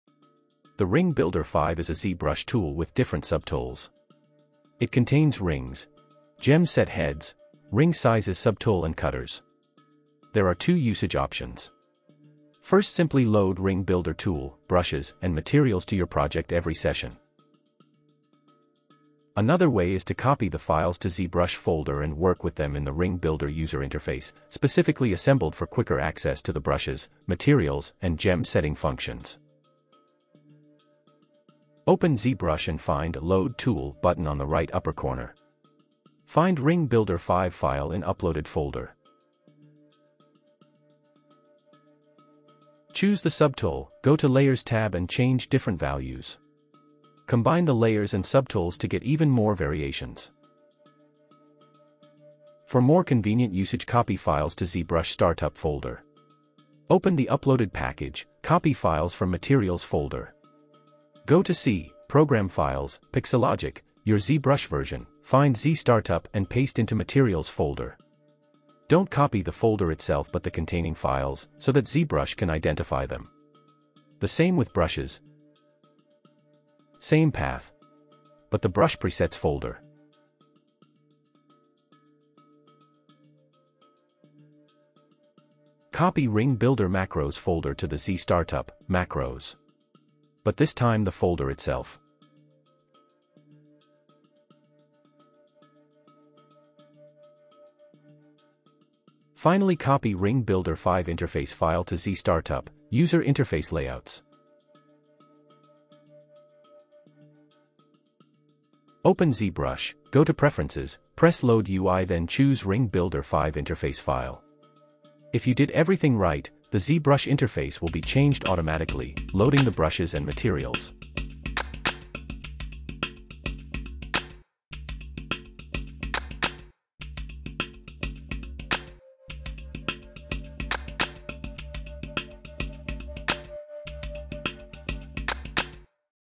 Zbrush Ring Builder 5 installation tutorial
The Ring Builder 5 is a ZBrush tool with different subtools. It contains rings , gem set heads, ring sizer subtool and cutters.

There are 2 usage options. First simply load Ring Builder tool, brushes and materials to your project every session. Another way is to copy the files to Zbrush folder and work with them in the Ring Builder user interface, specifically assembled for quicker access to the brushes, materials and gem setting functions.

Open ZBrush and find “Load tool” button on the right upper corner. Find Ring Builder 5 file in uploaded folder.

Choose the subtool, go to layers tab and change different values. Combine the layers and subtools to get even more variations.

For more convenient usage copy files to ZBrush startup folder. Open the uploaded package, copy files from Materials folder. Go to C/program files/pixsalogic/your Zbrush version/ find Zstartup and paste into Materials folder. Don’t copy the folder itself but the containing files. So that ZBrush can identify them.

The same with brushes, same path, but the Brush presets folder. Copy Ring Builder Macros folder to the Zstartup, macros, but this time the folder itself.

 Finally copy Ring Builder 5 interface file to Zstartup, User Interface Layouts. Open ZBrush, go to Preferences, press Load UI then choose Ring Builder 5 interface file. If you did everything right, the ZBrush interface will be changed automatically, loading the brushes and materials.

For using the brushes you should bake the layers, because you can not edit geometry with layers in ZBrush. Delete subdivision levels as well.

Also use 2021 and upper versions.